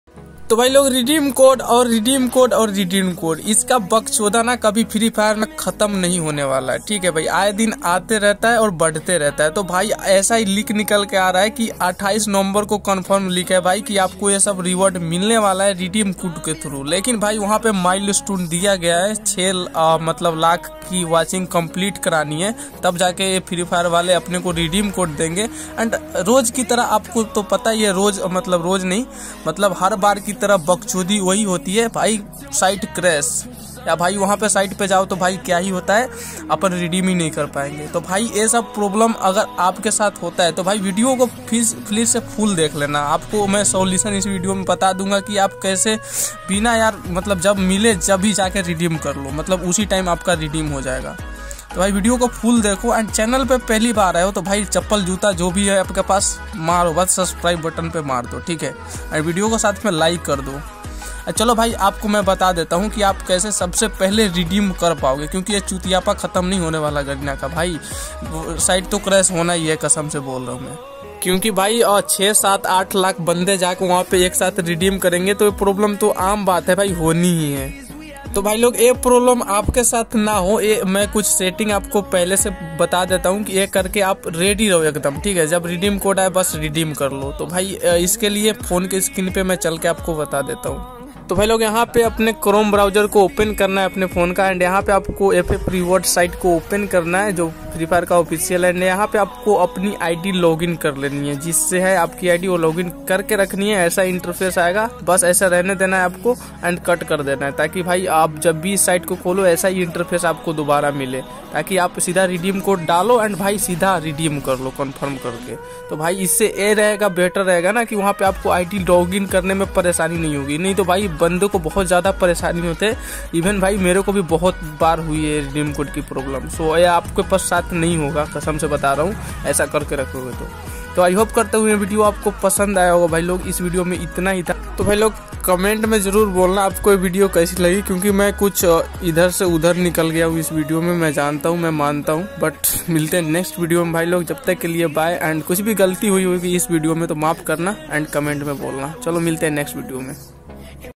28 Now को आने वाला Redeem Code Ko को सबसे पहले Redeem कैसे करे 🤔 Free Fire Redeem Code Site Problem
28 Now को आने वाला Redeem Code Ko को सबसे पहले Redeem कैसे करे 🤔 Free Fire Redeem Code Site Problem

Keep Supporting 🙏
Keep Loving ❤
Keep Sharing ⤴